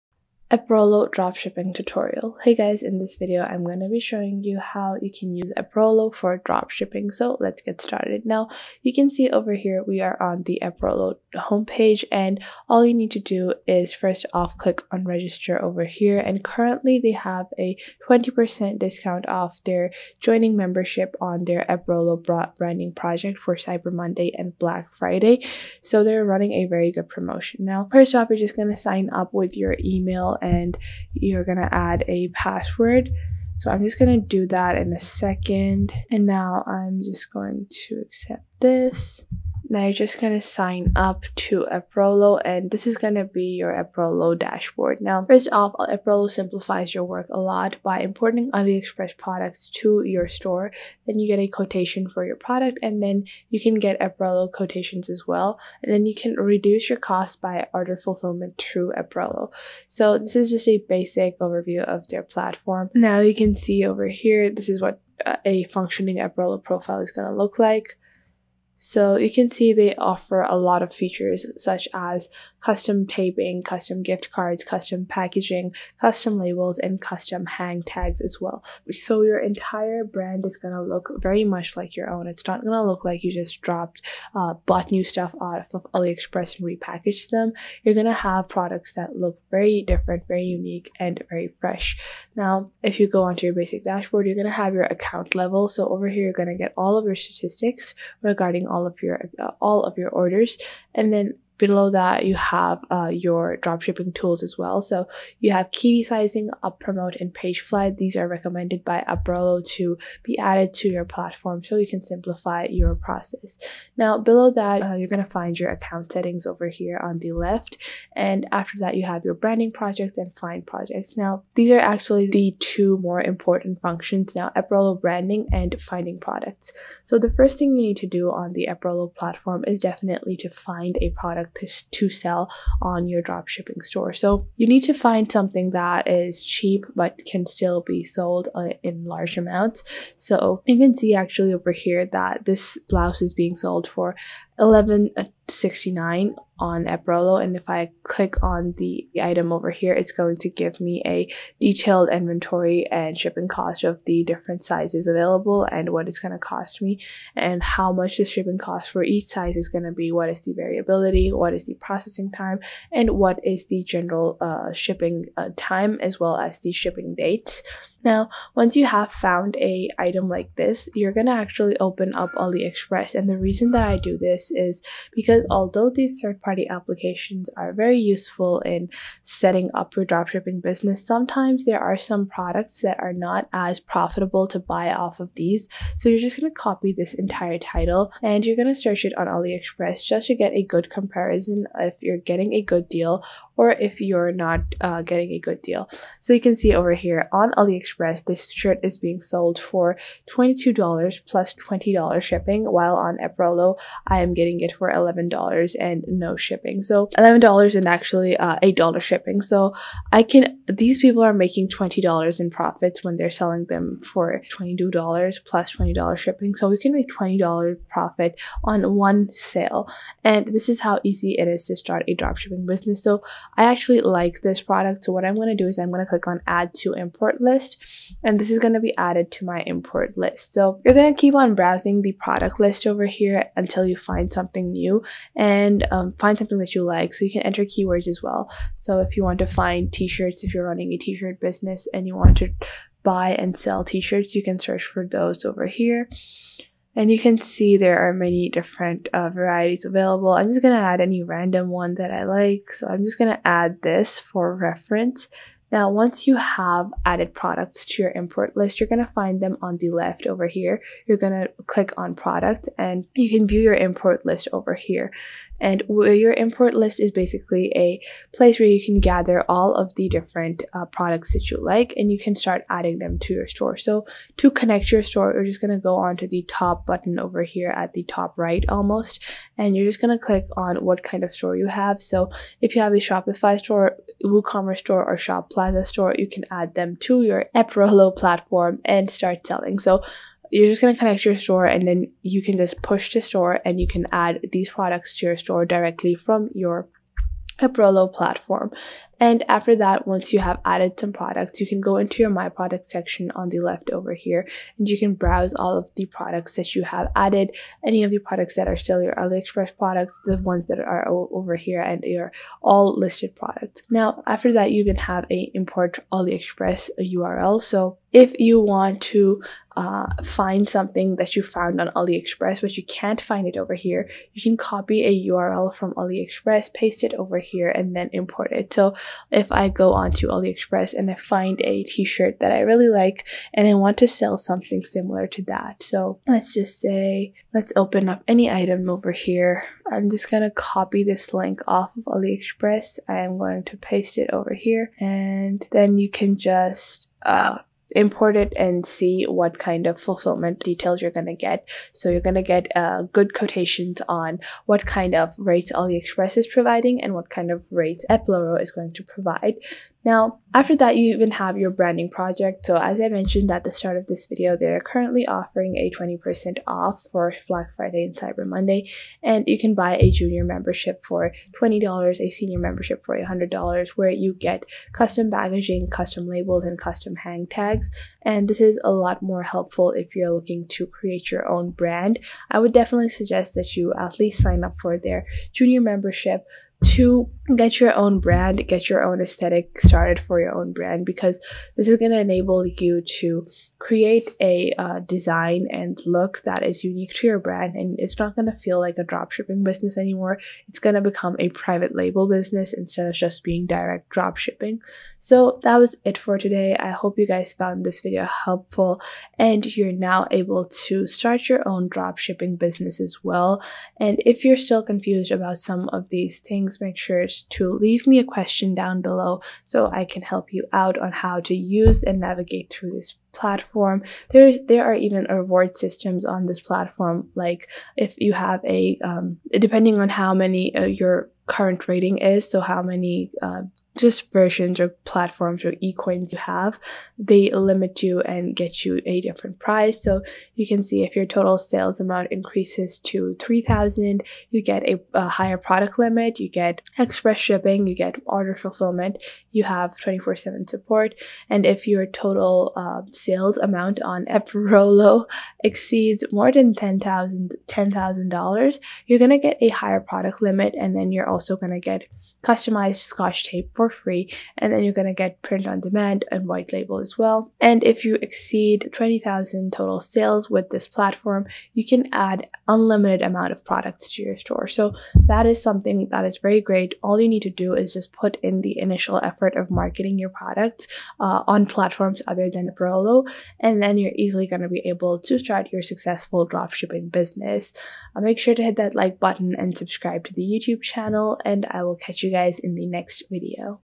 Eprolo Dropshipping Tutorial | How to Use EPROLO for Dropshipping
In this video I show you How to Use Eprolo , How does Eprolo works and What features Eprolo offers to help you with your dropshipping business.